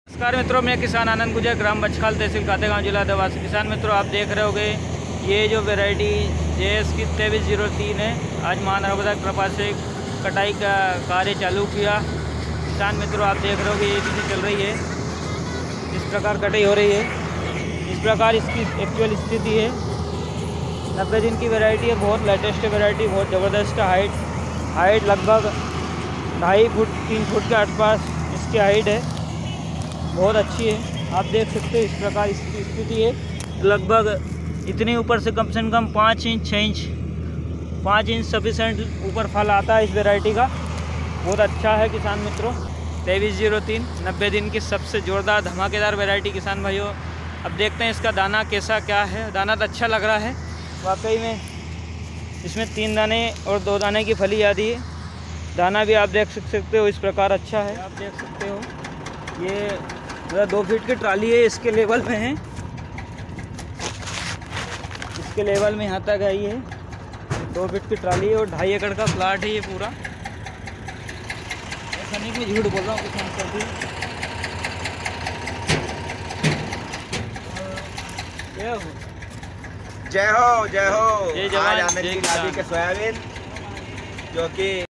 jS-2303 सोयाबीन बढ़िया ऐवरेज
आनंद गुजर खातेगांव से सोयाबीन 2303 बढ़िया एवरेज